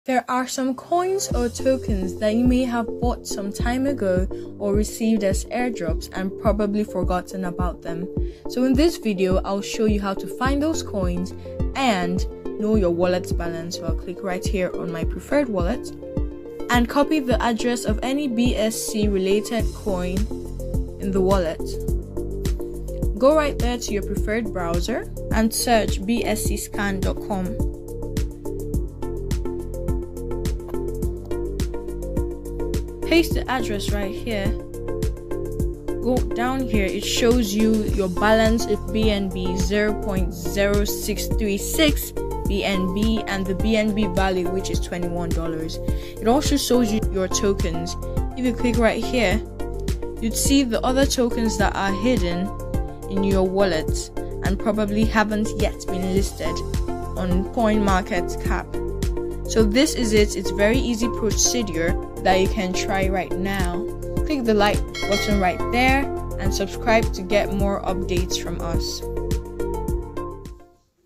How To Find Hidden Tokens And Wallet Balance Using BSCscan
👇 Here are the timestamps to navigate through the guide: 👇

0:00 - Introduction to Finding Forgotten Coins/Tokens
0:09 - How to Access Your Wallet Address
0:19 - Checking Balances with BscScan
0:58 - Conclusion and Call to Action

💡 About This Video: Ever wonder if you have forgotten coins in your wallet? This quick tutorial guides you through finding and checking the balance of coins and tokens that might have slipped your memory, using BscScan.com. Learn to track your assets efficiently and discover potential hidden treasures in your crypto wallet.